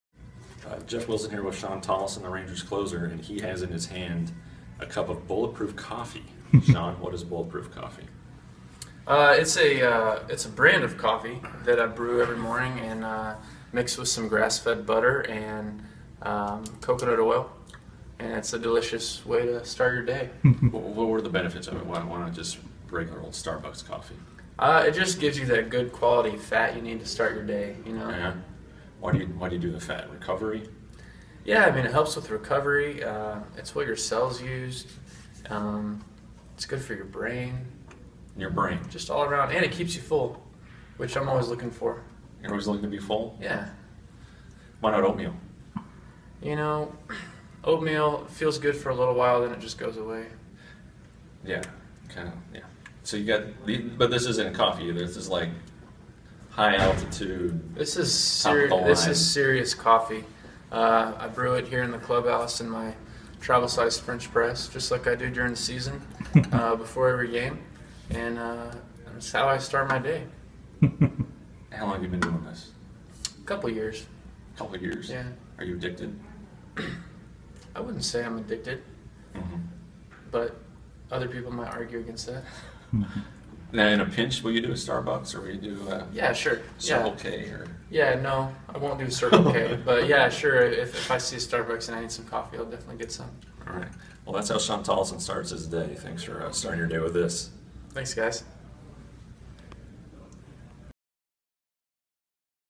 Rangers closer Shawn Tolleson feels "Bulletproof"
Bulletproof Coffee helps get Shawn Tolleson going each spring morning (video by Jeff Wilson).